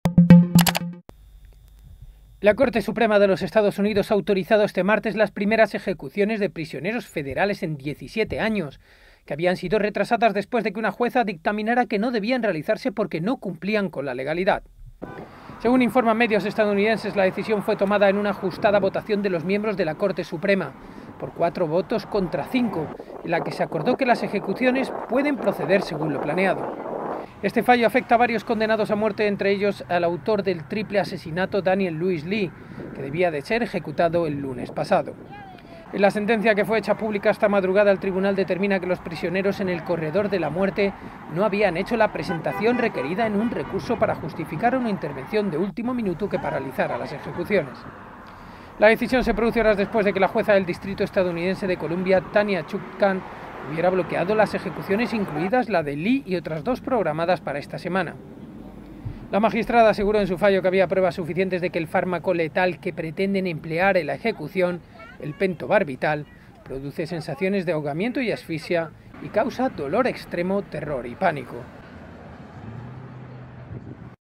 Corte Suprema de EUA autoriza las primeras ejecuciones federales en 17 años
La Corte Suprema de Estados Unidos ha autorizado este martes las primeras ejecuciones de prisioneros federales en 17 años, que habían sido retrasadas después de que una jueza dictaminara que no debían realizarse porque no cumplían con la legalidad.

¡Visítenos!